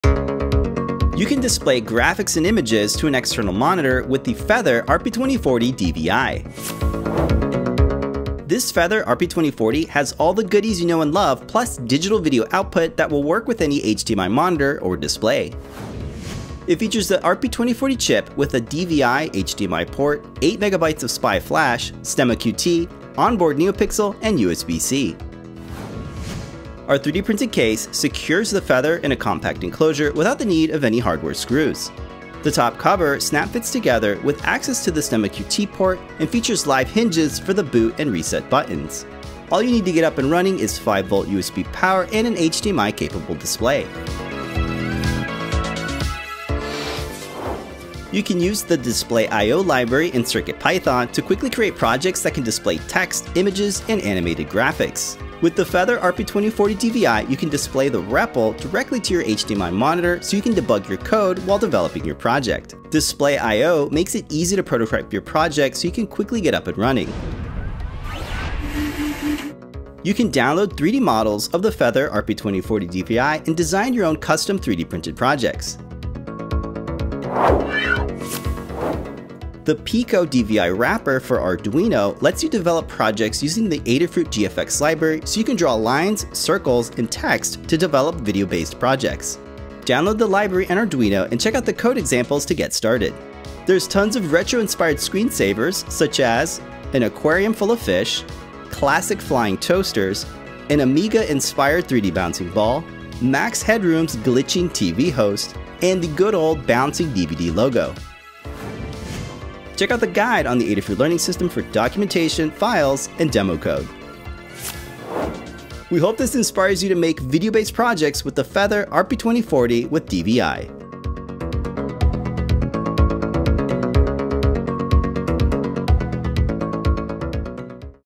HDMI/DVI images on a monitor
Use HDMI/DVI to display graphics and images to an external monitor with the Feather RP2040 DVI